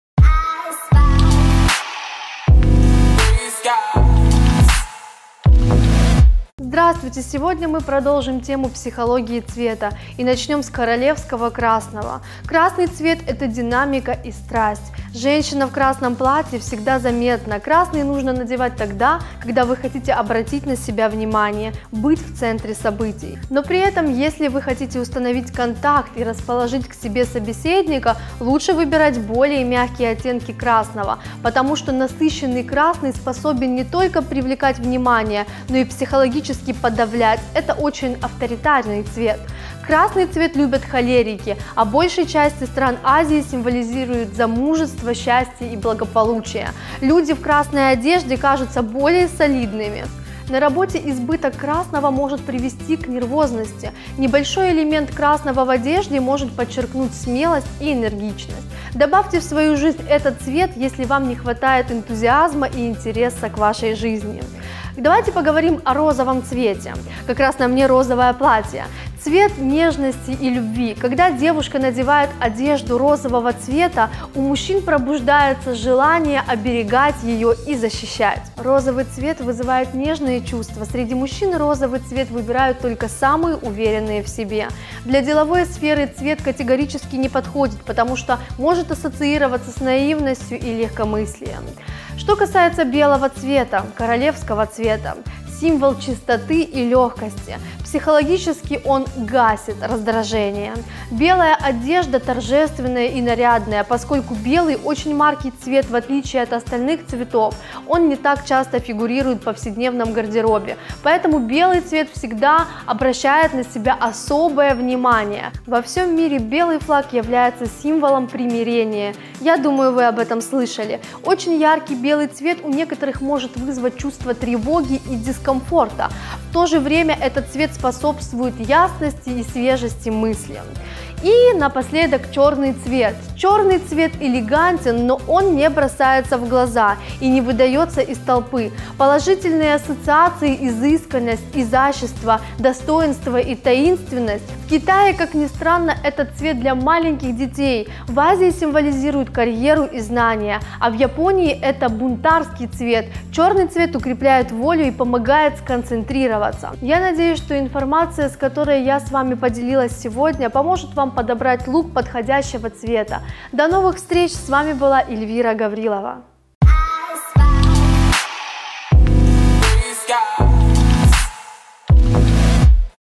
Что значат цвета в одежде и как это использовать by Elvira Gavrilova, часть 2
В продолжение темы о психологии цвета сегодня поговорим о страстном красном, королевском белом иии...? Смотрите видео, чтобы разобраться в оставшихся цветах и их значениях, а также как следует носить вещи определенного цвета.

Моя группа Facebook "Elvira Gavrilova" - украинский бренд одежды. Самые нежные и утонченные модели вечерних платьев 👗